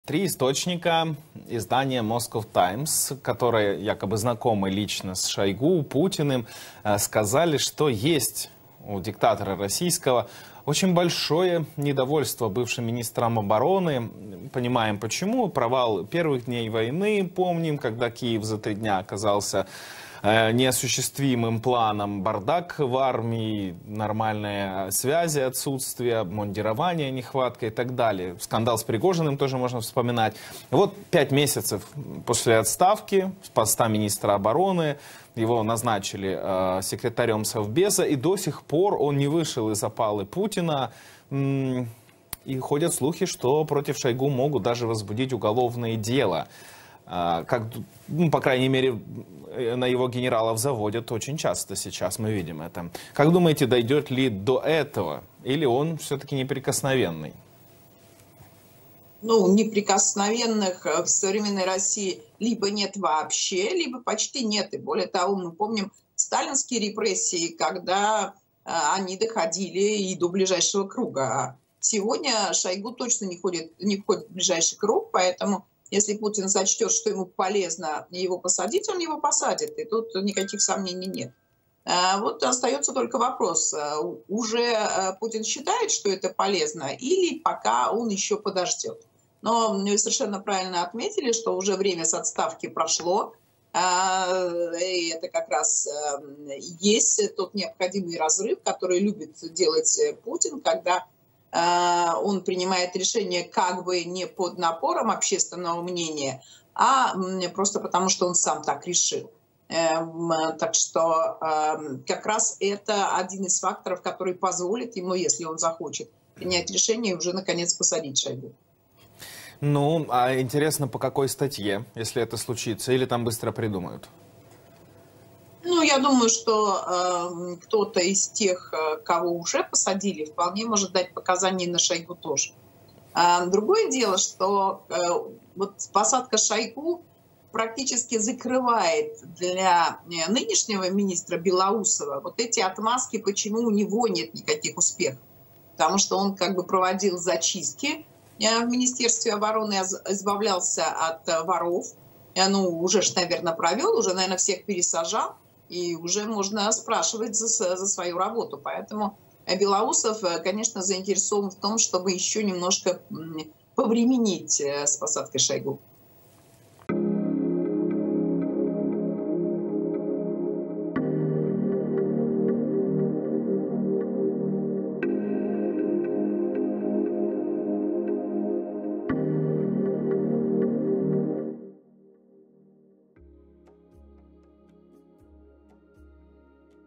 Зачистка в Москве: Шойгу — на очереди! Дружка Путина отправят за решетку
RU - Одним из следующих в очереди на арест может стать экс-министр обороны РФ Шойгу. Почему - объясняет политический аналитик Ольга Курносова.

UA - Одним із наступних у черзі на арешт може стати екс-міністр оборони РФ Шойгу. Чому – пояснює політичний аналітик Ольга Курносова.